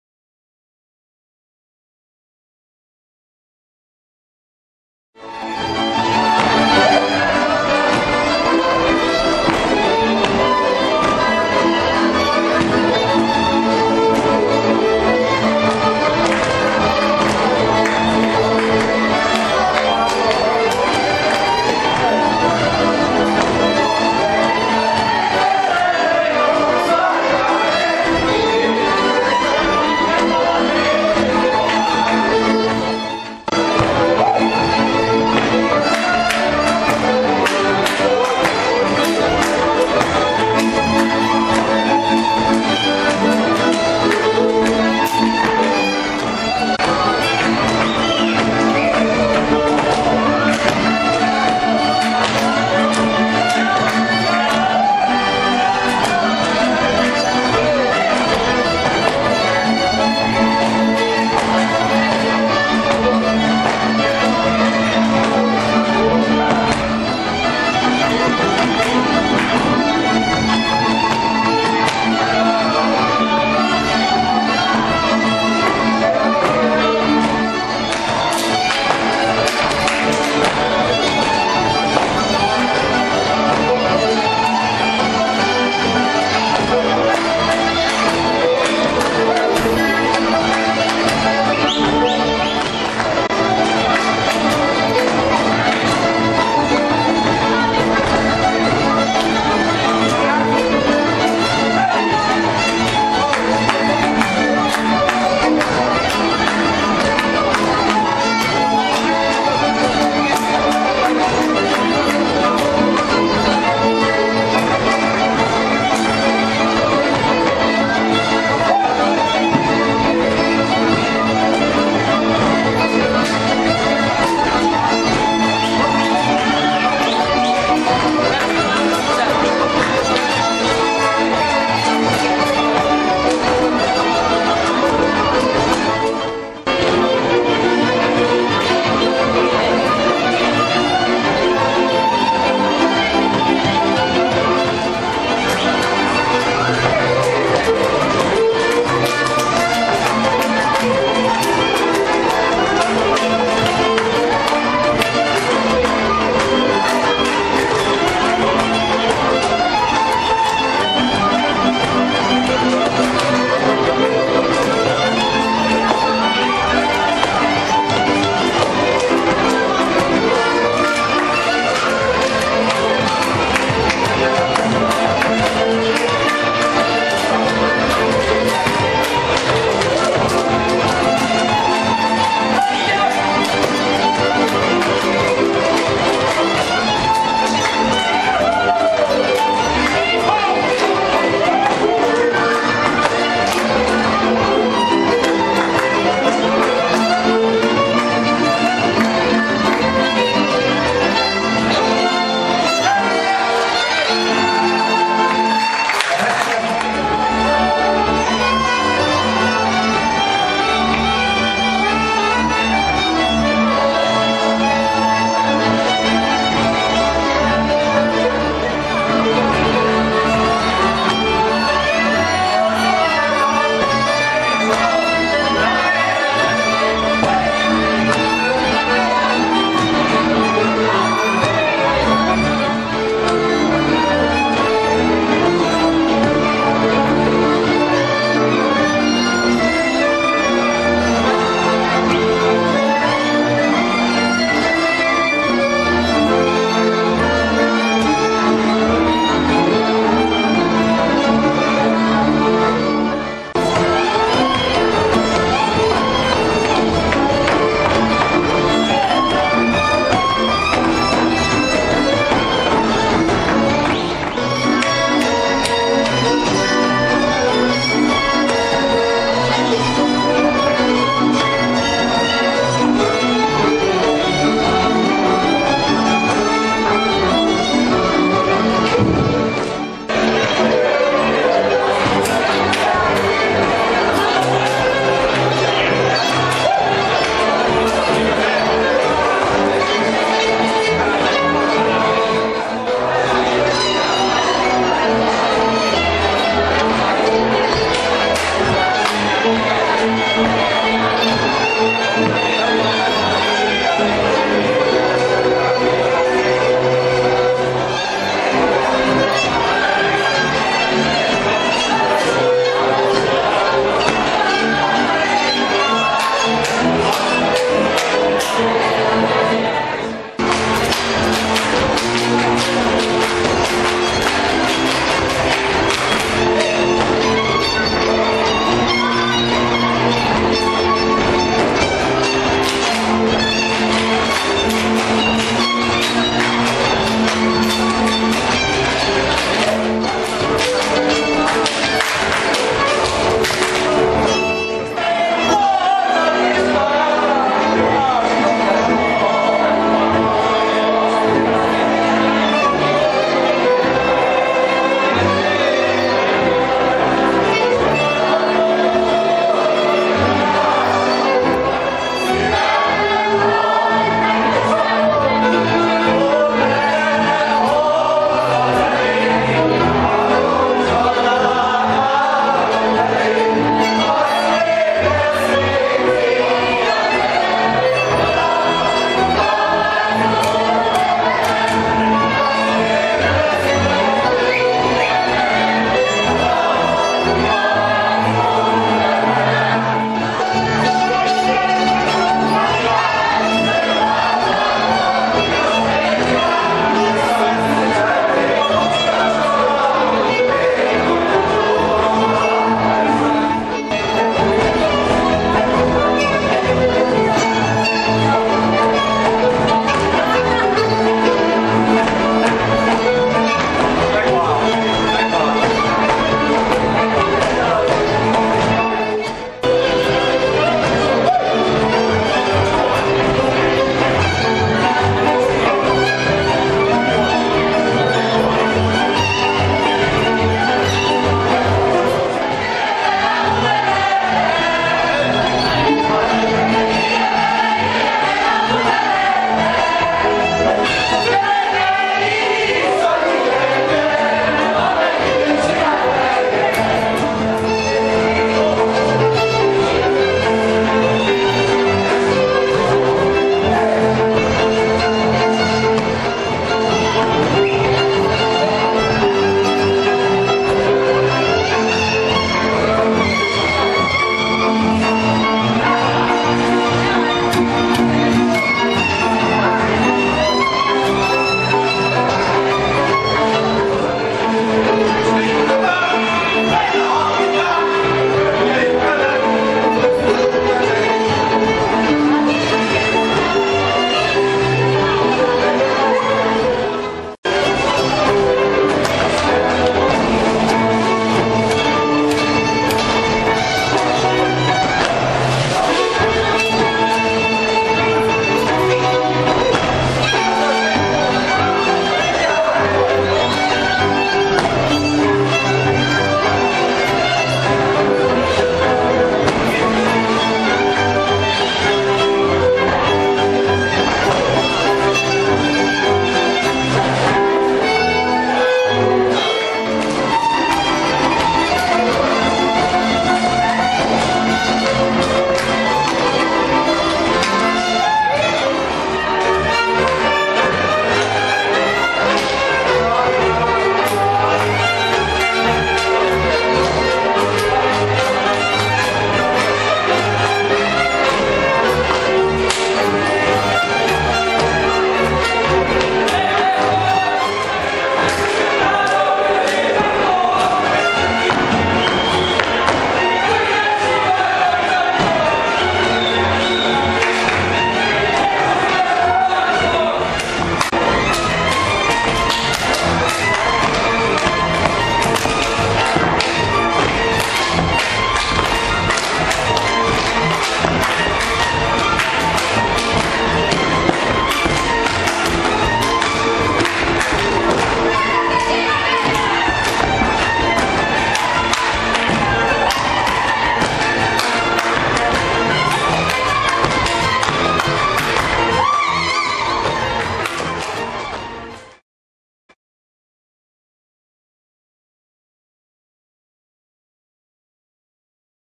Székelyföldi 2008: Part 9 Video Clips
Video clips (Part 9 of 16) from the 14th Székelyföldi Folk Dance Camp in Felsősófalva, Erdély (Transylvania - Ocna de Sus, Romania); July 20-27, 2008. (with high quality option)